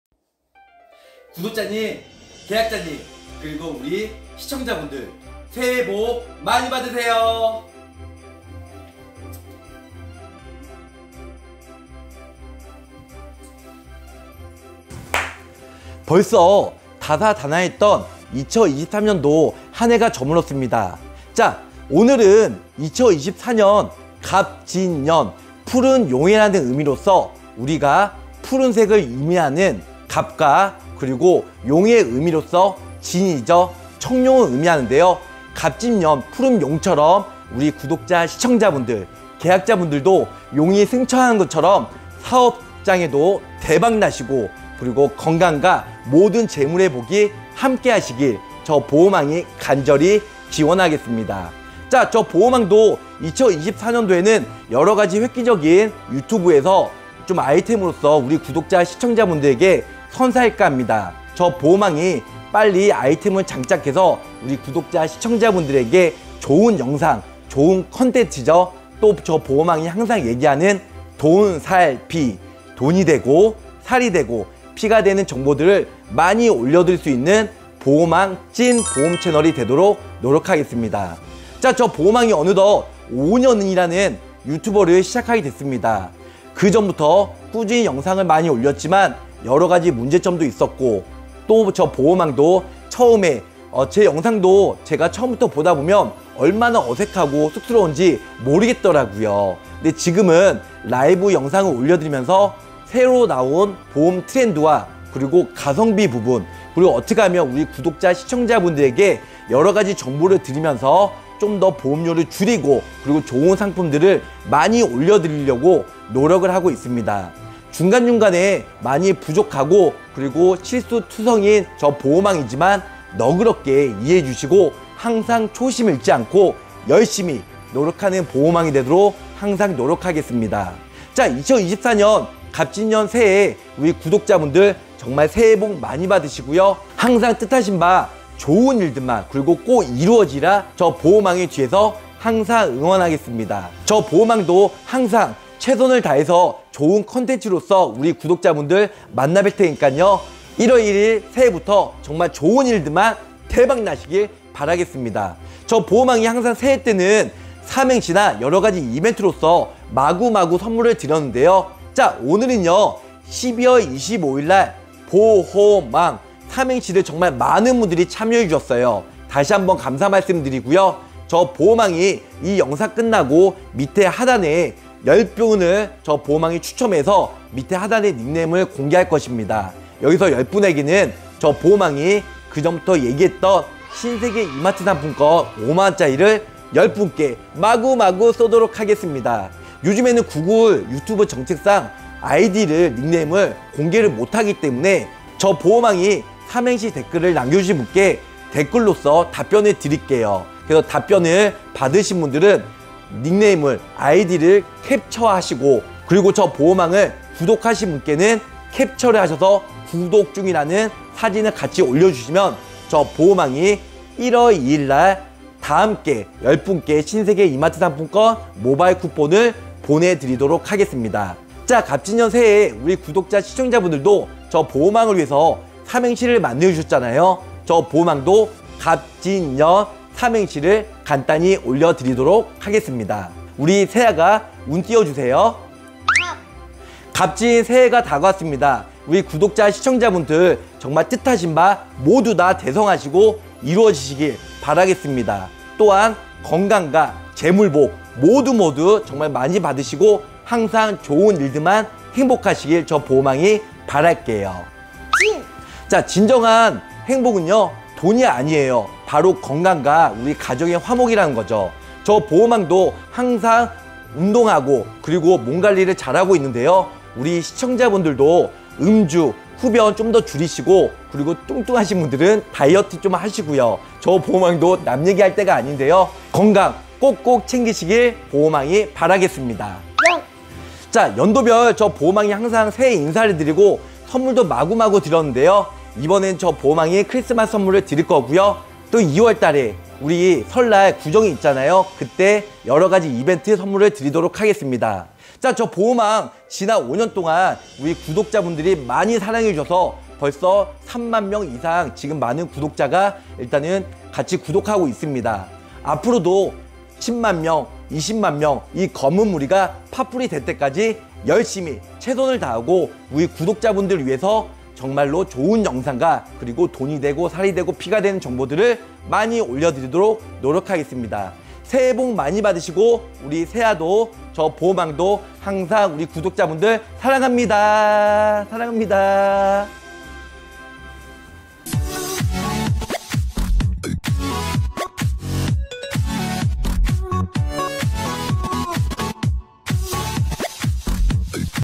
2024년 갑진년 새해 복 많이받으세요~♡♡ f.보험왕삼행시 이벤트 당첨자발표!!
2024년 새해복많이받으세요!!
갑진년 새해 뜻하신 모든 일들이 이루어지시길
저 보험왕이 간절이 바랍니다

24년 올해에도 좋은 컨텐츠로 찾아뵐것을

크리스마스 보험왕삼행시 당첨자
발표하겠습니다!!

닉네임 호명되신분들께서는 개인적으로
저에게 카톡이나 문자로 답변주시고

구독중이라는 화면과 닉네임확인할수있는
사진을 캡쳐하여 보내주시면 되겠습니다

진심으로 축하드리며 다시한번
새해복많이받으세요!!

당첨자 명단
1.구연선
2.상옥송
3.suk won L
4.난다김
5.스트라이트
6.철
7.이의용
8.happy day0909
9.익관장
10.hwa sun kim

이상 10분께 신세계 모바일상품권
1월2일 일괄적으로 보내드리겠습니다

보험은 언제나 보험왕에게!!